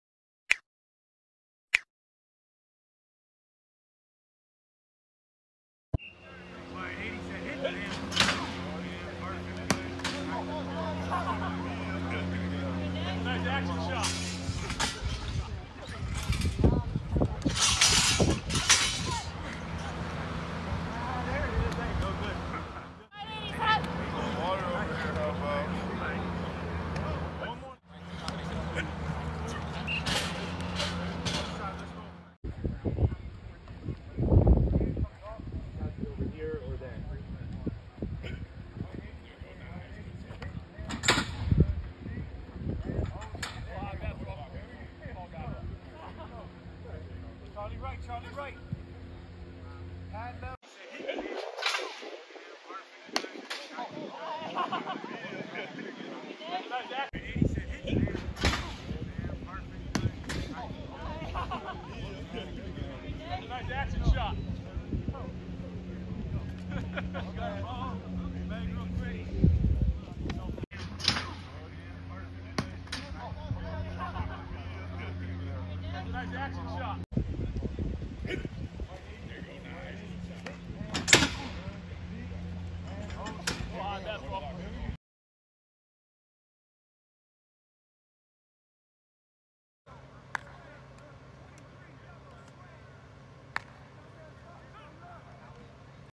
First Look Baltimore Ravens Jason Pierre-Paul LOOKS DOMINANT 🔥
CREDIT: Jamison Hensley, WJZ Sports, Melissa Y. Kim, Ryan McFadden, Bo Smolka, Pete Gilbert, Ravens